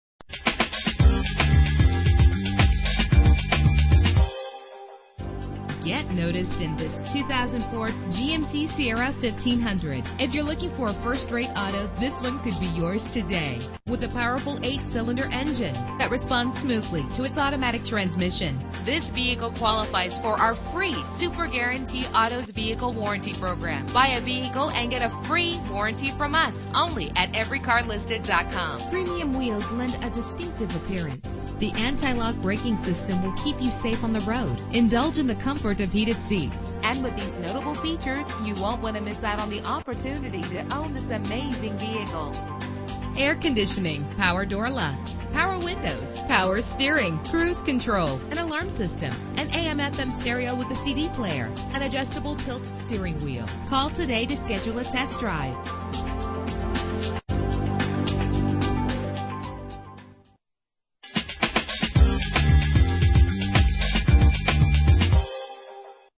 2004 GMC Sierra 1500 - Grandberry PA
Year: 2004
Make: GMC
Model: Sierra 1500
Trim: SLE
Engine: 5.3 liter 8 cylinder 16 valve
Transmission: 4-Speed Automatic
Color: Gray
Mileage: 98783
Address:
7082 US 322
Grandberry, PA 16319